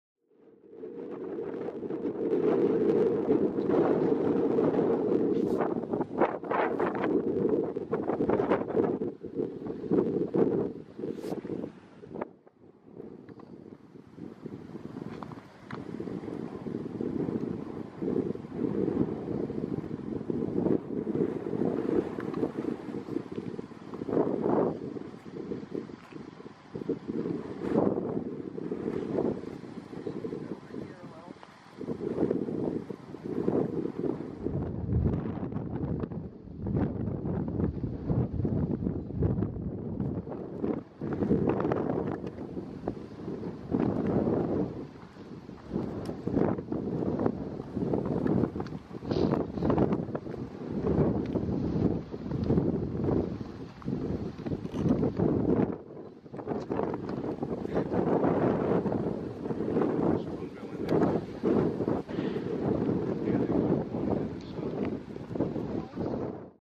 Shorebirds & Waders
Shorebirds and wading birds at Black Point Nature Drive northwest of the Kennedy Space Center.